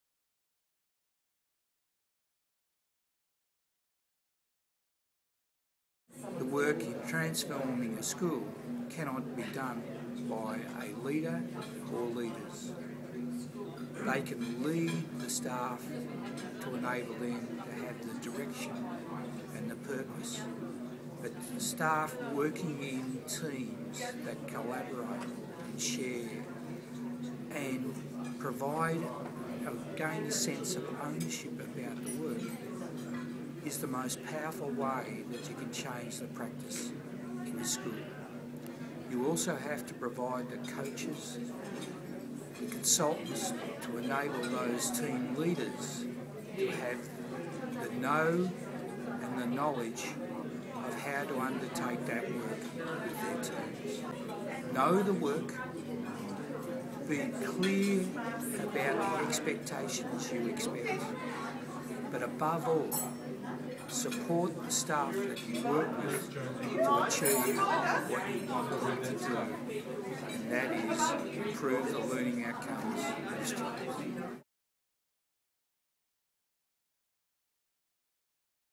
Learning Conversations
Colin Schot, Principal at Warringa Park School, Werribee talks about how to lead transformation in schools at Catholic Education Diocese of Parramatta's most recent Principals' Masterclass.